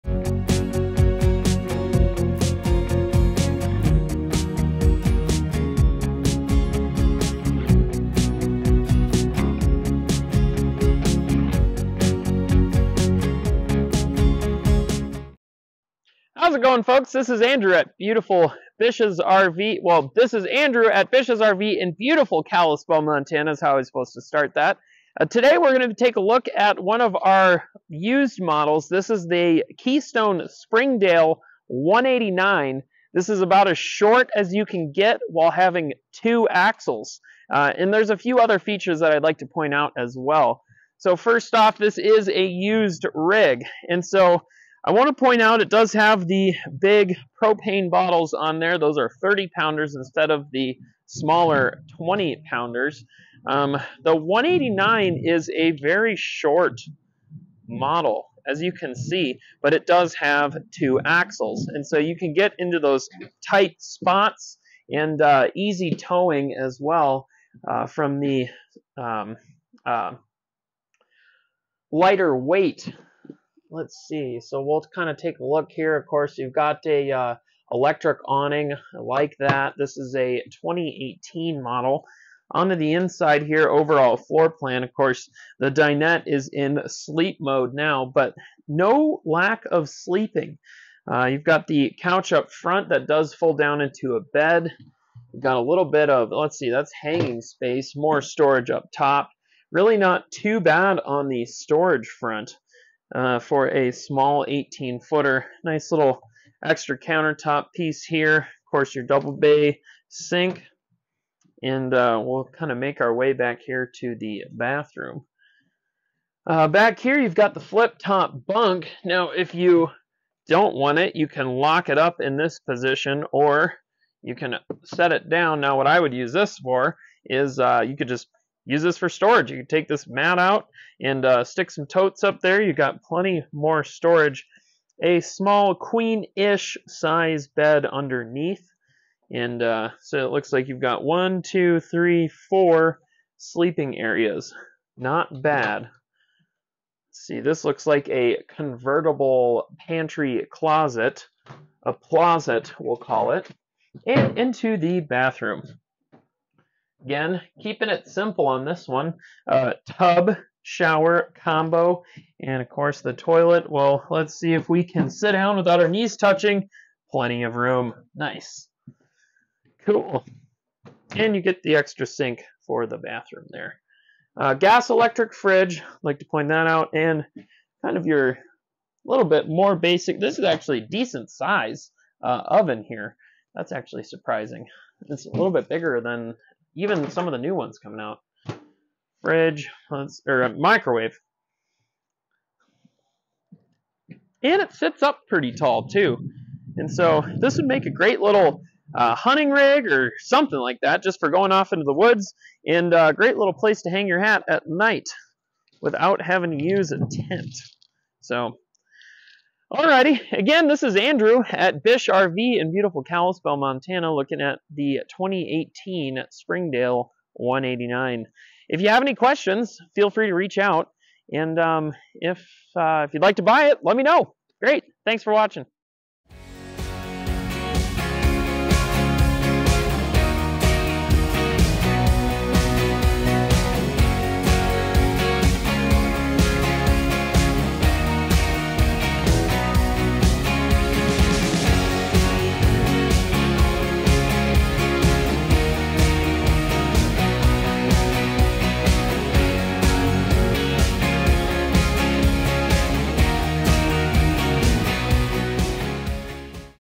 2018 Keystone Springdale 189 FLWE Travel Trailer • Bishs.com
If you're in the market for a Travel Trailer, this 2018 Keystone Springdale 189FLWE could be what you're looking for. Rest easy in this Travel Trailer which comfortably sleeps 7. And contains a Bunkhouse. There's room for everyone and everything. Conveniently located in this Springdale Travel Trailer are a Center kitchen with Bench Seats, Front living area and Rear bathroom. The Springdale 189FLWE Travel Trailer make on-the-road living easy with features like a stove with 3 oven burners, Mid-Size refrigerator, and more!